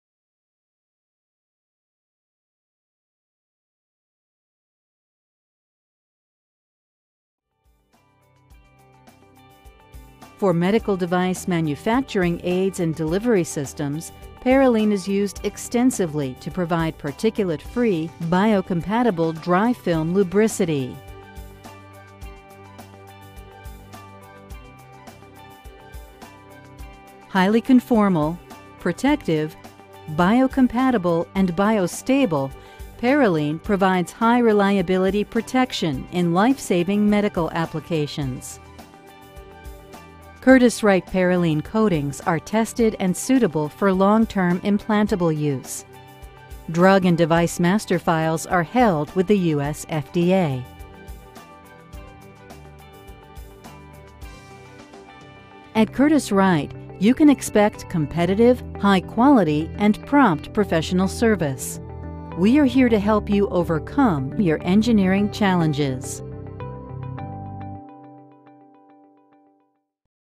Medical Device Parylene Conformal Coating with Parylene
Medical devices and instruments often require FDA compliant coating protection that is fully conformable, biocompatible and bio stable. Inert, vacuum deposited Parylene film meets rigorous medical coating requirements more effectively than any other conformal coating, making it the long-standing material of choice for items ranging from needles and catheters to epidural probes, medical device circuits, and mandrels.